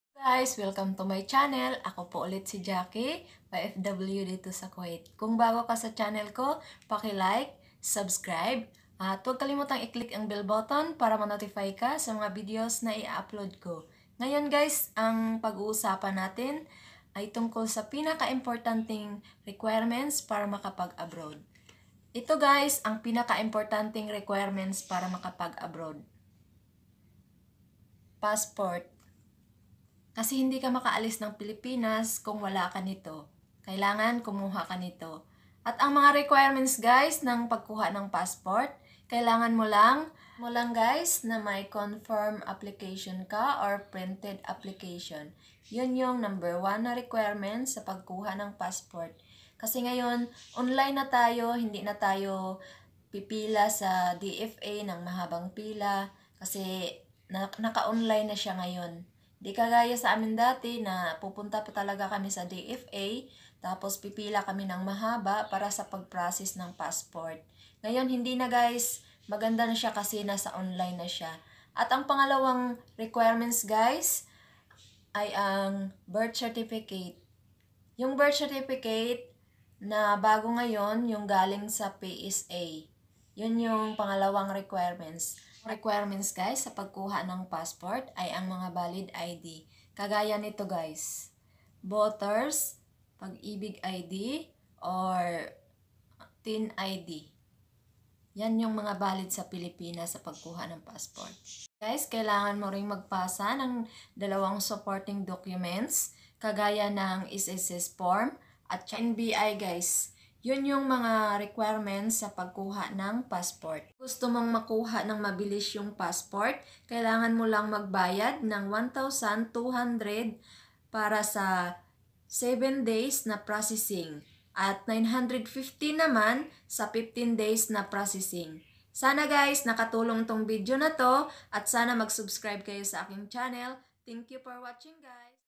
Passport requirements for abroad Kuwait ofw vlog
Ang video na ito Passport requirements for abroad ginawa ko para makatulong ako sa iba na gustong mag abroad ng sa ganon malaman nila na passport ang isa sa mga Requirements for abroad.